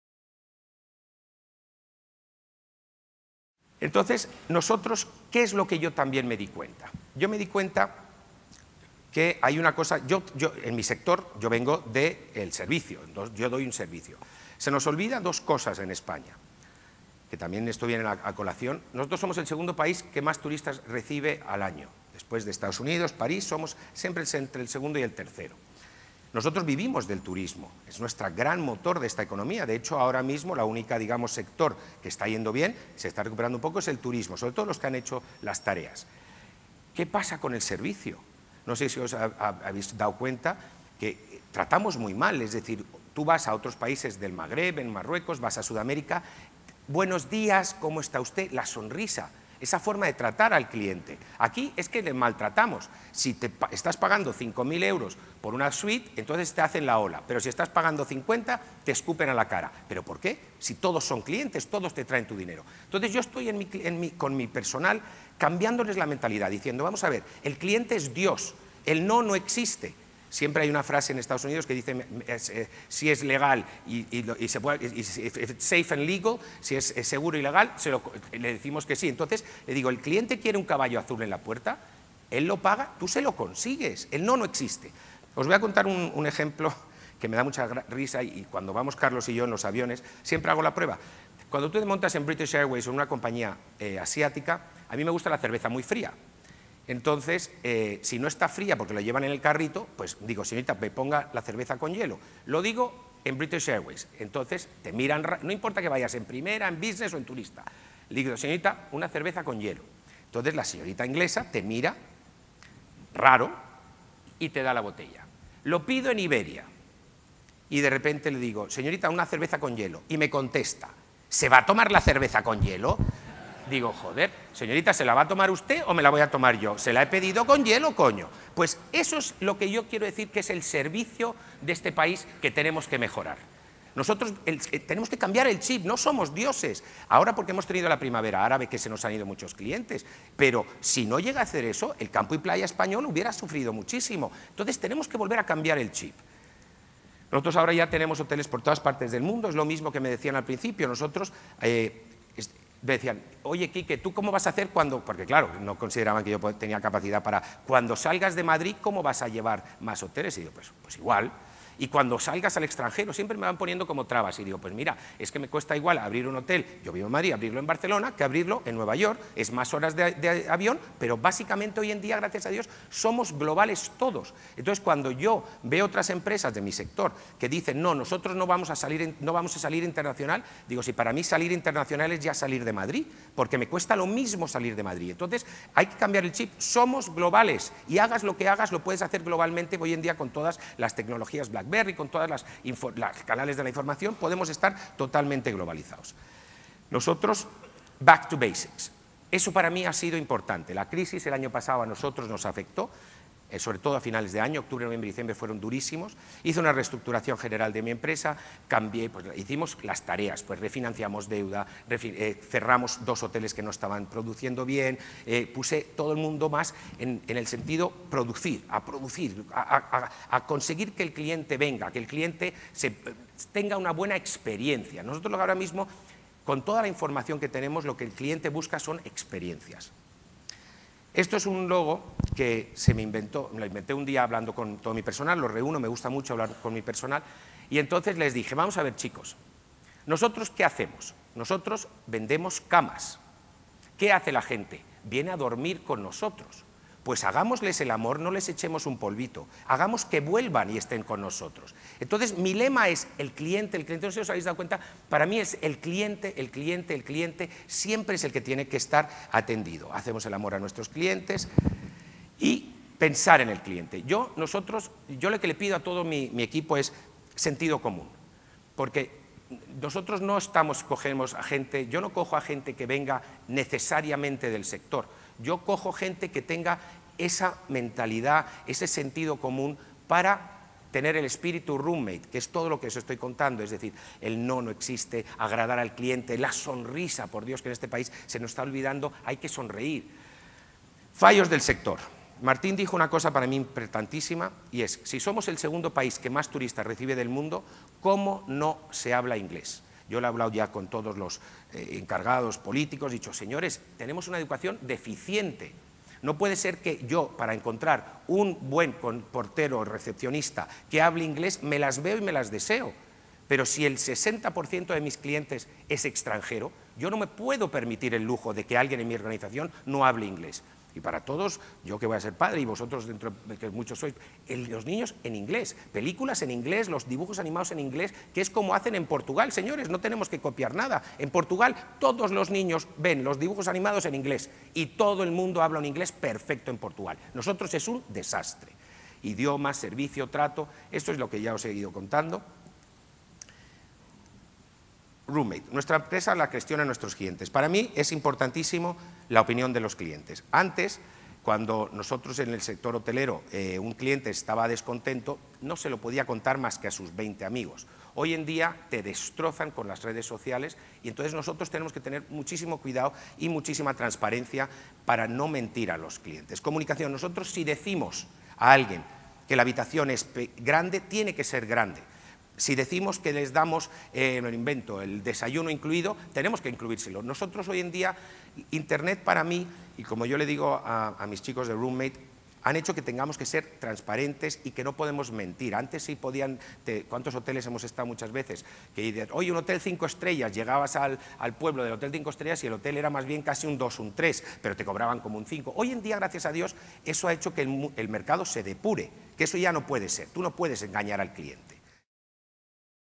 Ponencias del BlackBerry Innovation Forum 2011 Madrid 9
Parte 3 de 3:  Sesión plenaria presentada por Kike Sarasola, Fundador y Presidente de Room Mate Hotels

Ponencias impartidas por Eduardo Fernandez, Martin Varsavsky, Enrique Sarasola durante el BlackBerry Innovation Forum del 3 de noviembre del 2011 en Madrid, España